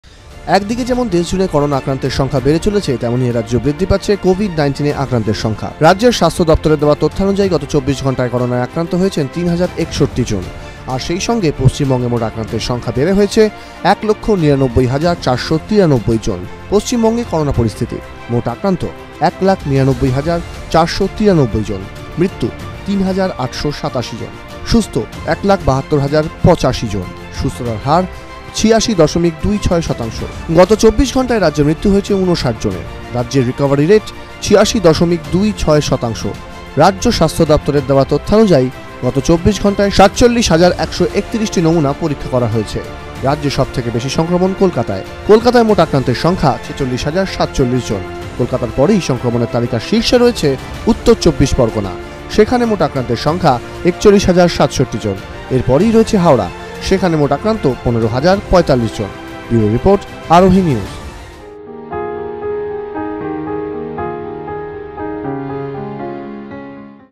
Covid-19 situation in state | কোভিড-১৯ UPDATE
Covid-19 situation in state | আক্রান্তের সংখ্যা প্রতিদিন বাড়লেও সুস্থ হয়ে উঠছেন বেশির ভাগ আক্রান্তই। রাজ্যে রিকভারি রেট ৮৬ শতাংশের বেশি।

@Aarohi News